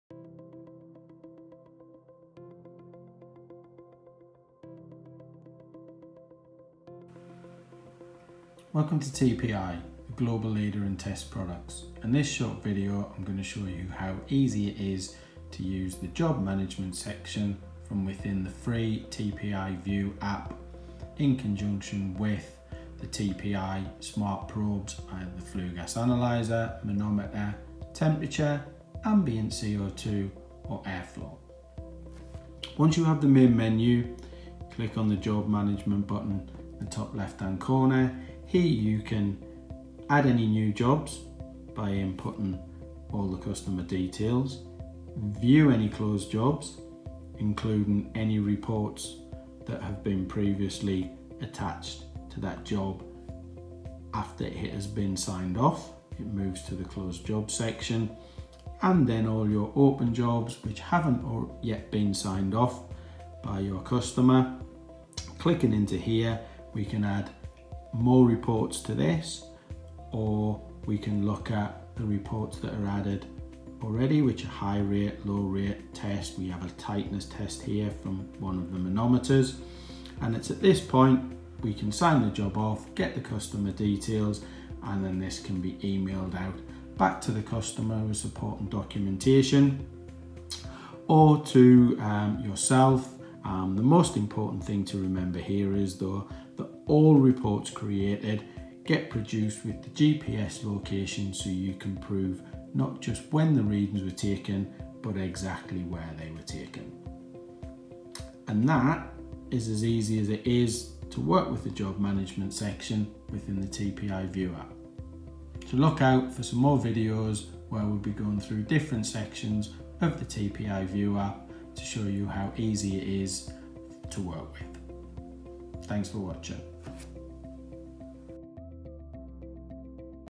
TPI View App and Smart Device Job Management
See how easy it is to use the Job Management feature of the TPI View app for smartphones and tablets with any of the TPI smart probes, manometers and the DC710 Flue Gas Analyser